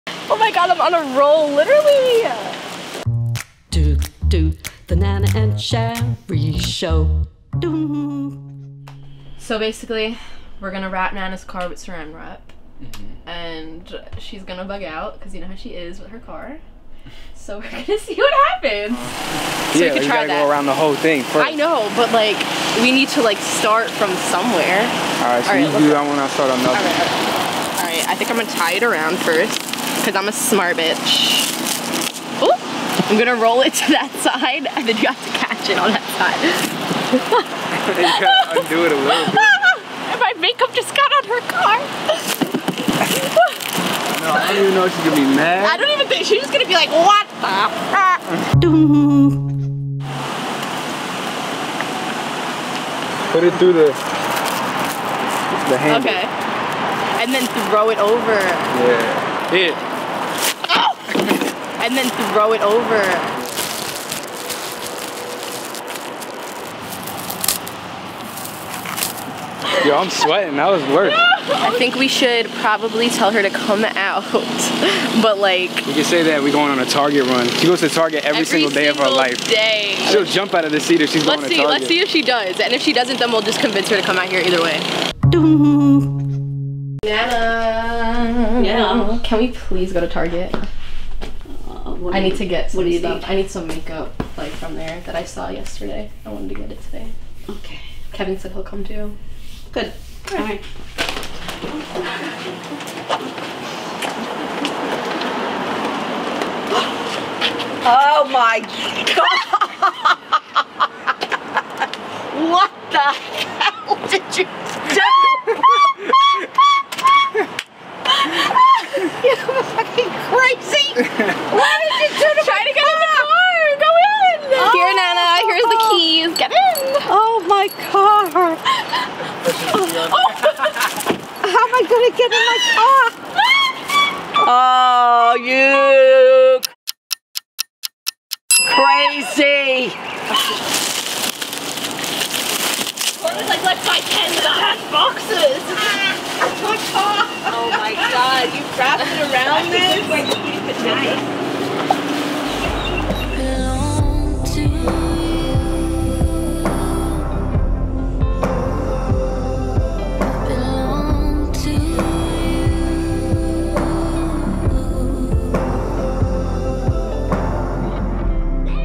Nana Gets Wrapped Up | The Shari Marie & Nana Show Ep. 4
On this episode of "The Shari Marie & Nana Show" Nana attempts to go to Target only to find that Shari & Kev had other plans for her.